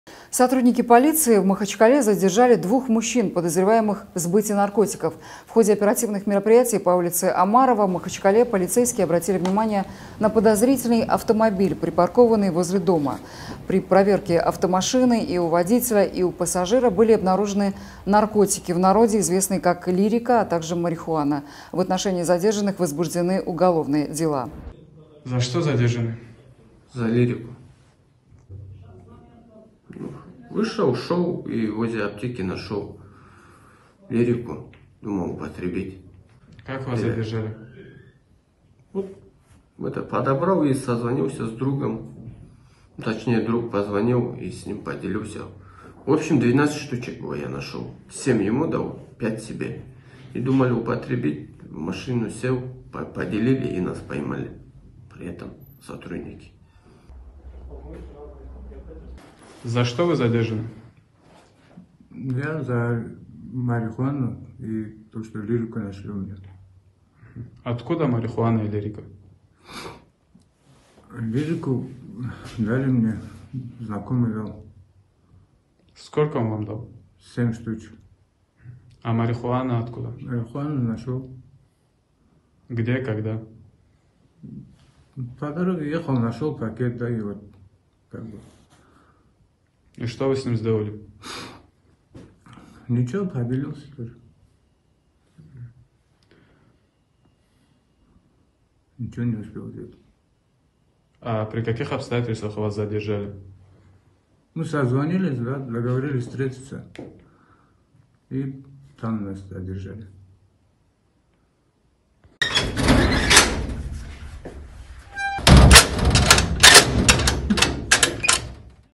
Полицейские задержали в Махачкале подозреваемых в сбыте наркотиков
Сотрудники полиции в Махачкале задержали двух мужчин, подозреваемых в сбыте наркотиков.

В ходе оперативных мероприятий по улице М.Омарова в Махачкале полицейские обратили внимание на подозрительный автомобиль, припаркованный возле дома. При проверке автомашины и у водителя, и у пассажира были обнаружены наркотики в народе известные как “Лирика”, а также марихуана.

В отношении задержанных возбуждены уголовные дела.